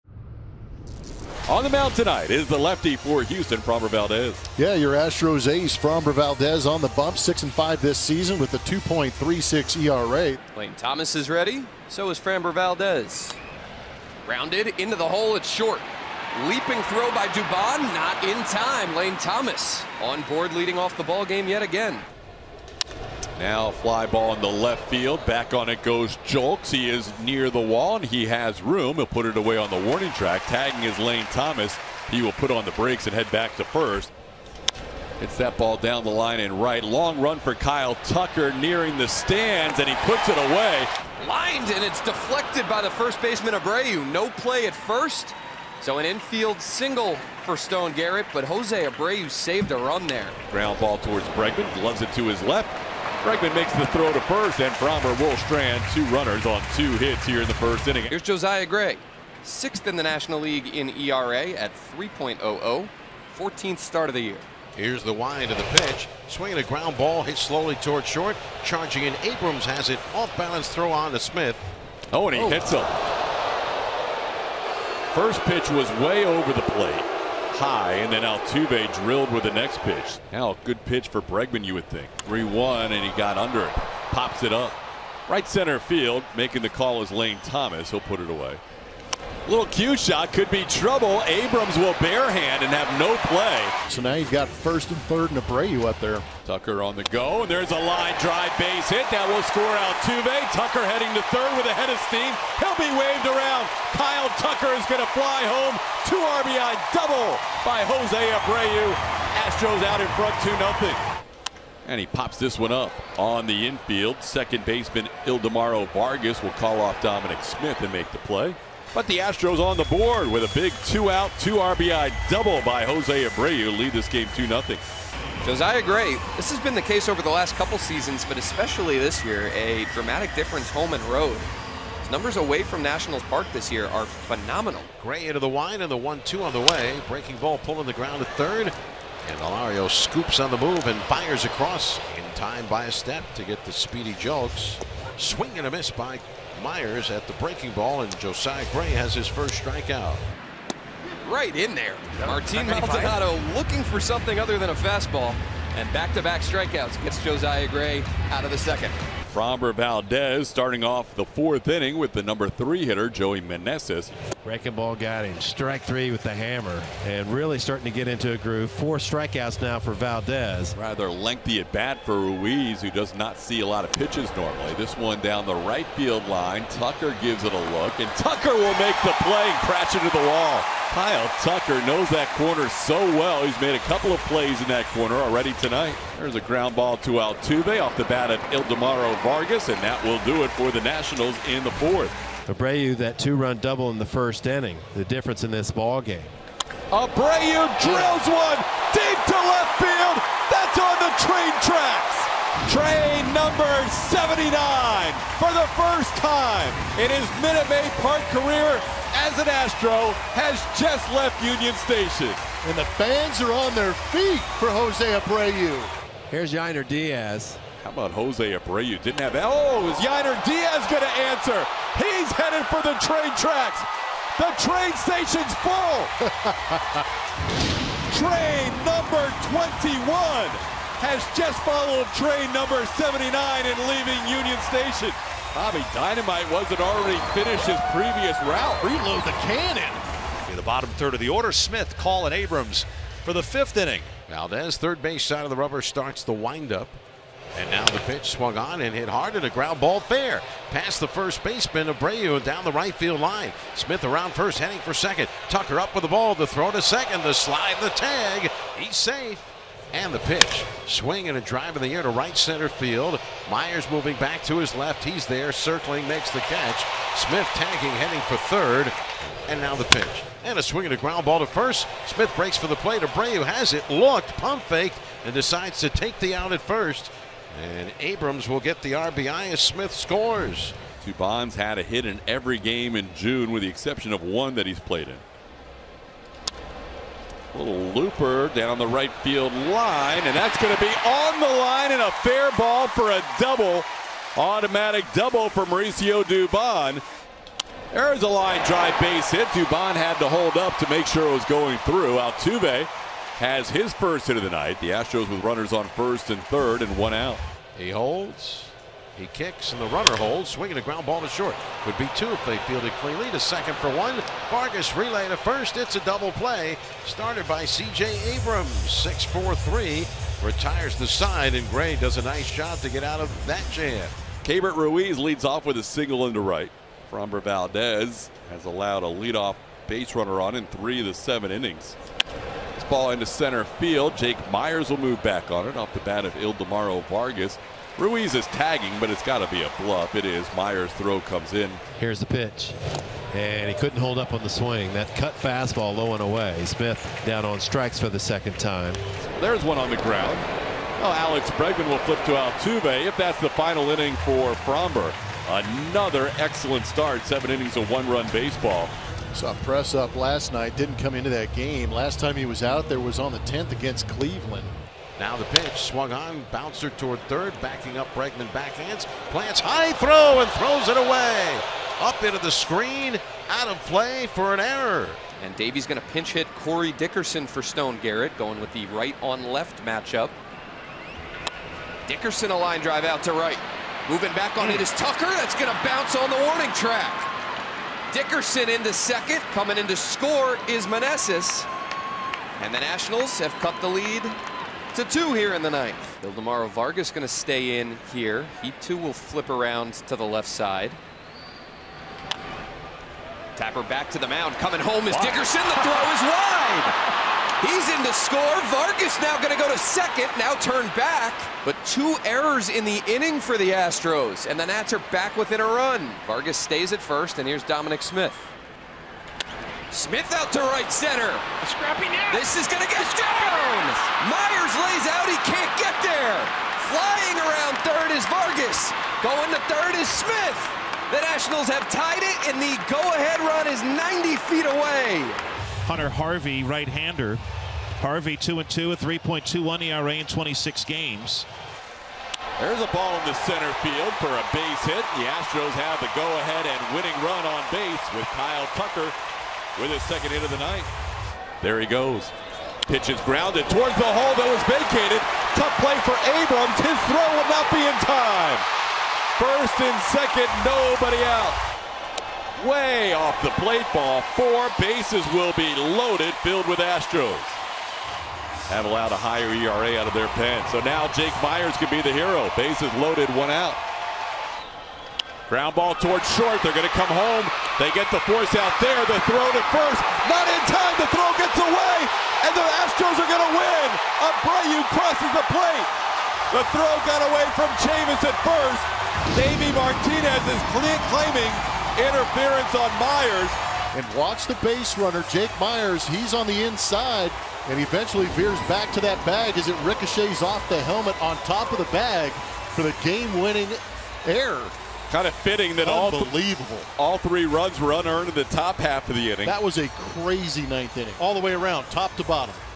Nationals vs. Astros Game Highlights (6/14/23) | MLB Highlights
Nationals vs. Astros full game highlights from 6/14/23